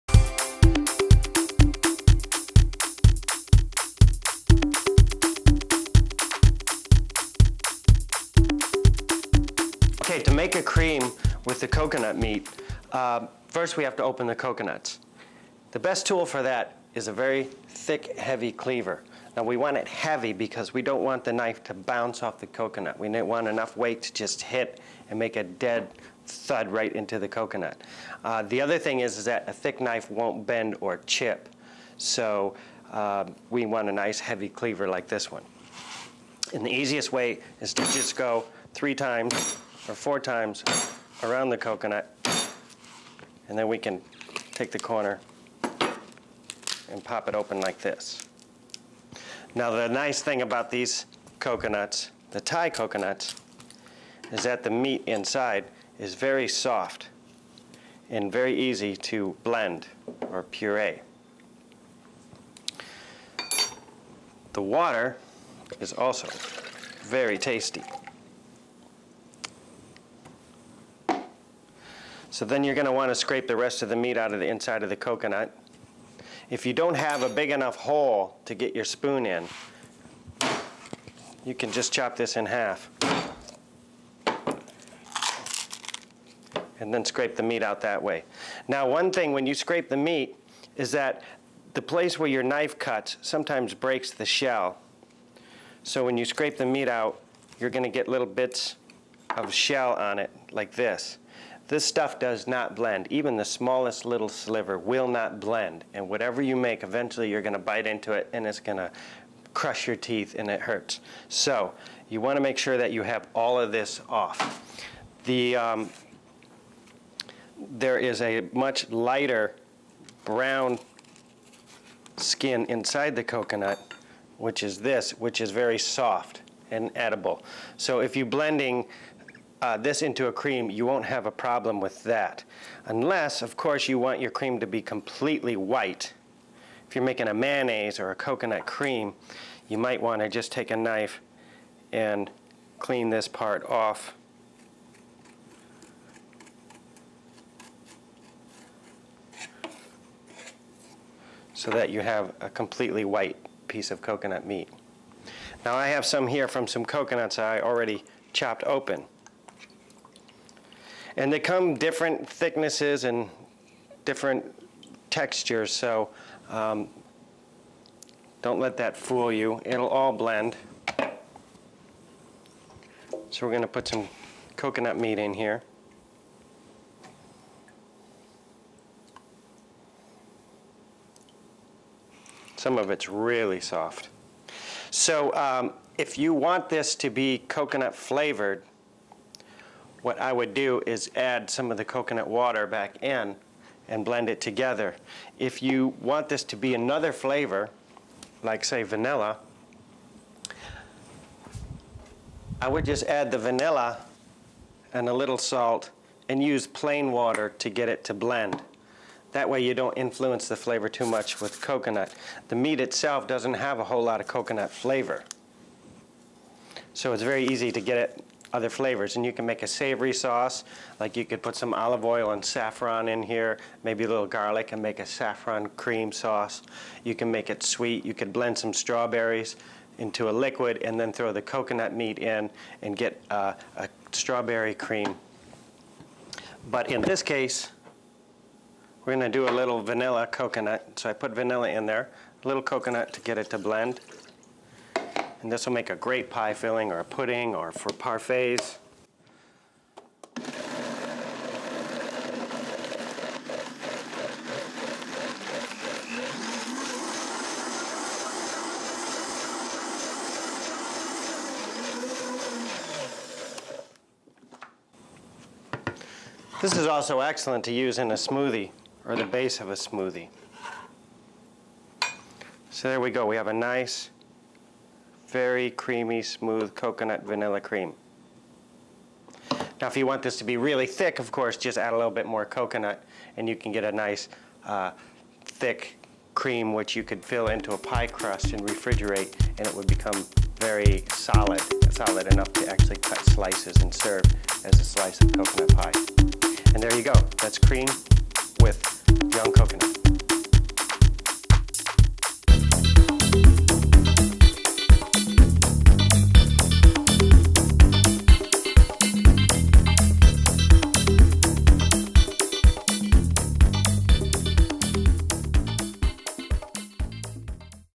Coconut Cream
How to make a cream from Young Coconut.